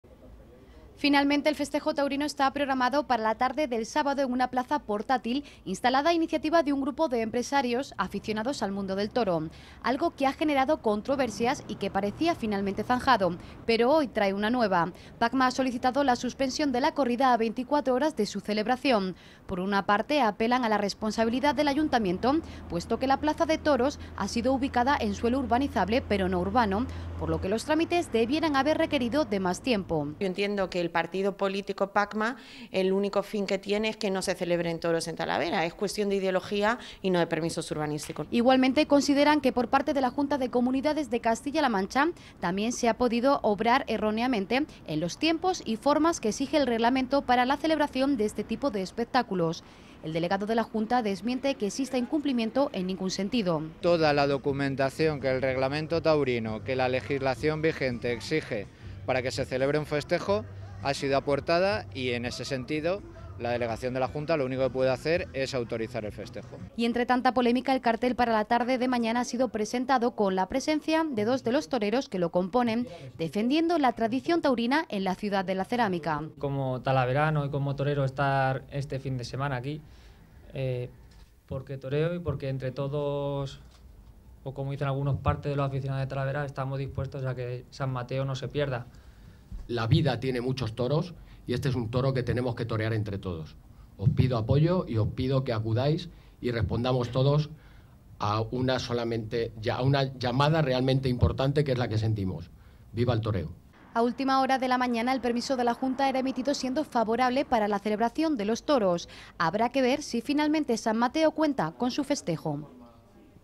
HABRÁ FESTEJOS TAURINOS EN TALAVERA DE LA REINA PESE A LAS DENUNCIAS DE PACMA
La polémica de los toros en Talavera continúa. Al final parece que sí tendrán festejo taurino. Está programado para mañana en una plaza portátil. Pero como bien dice el refrán nunca llueve a gusto de todos y el partido animalista Pacma denuncia presuntas irregularidades en la concesión de autorizaciones. Pacma habla de incumplimientos en la tramitación del permiso municipal según la normativa urbanística: la plaza portátil está en suelo urbanizable, lo que requiere de un procedimiento largo para la autorización de actividad.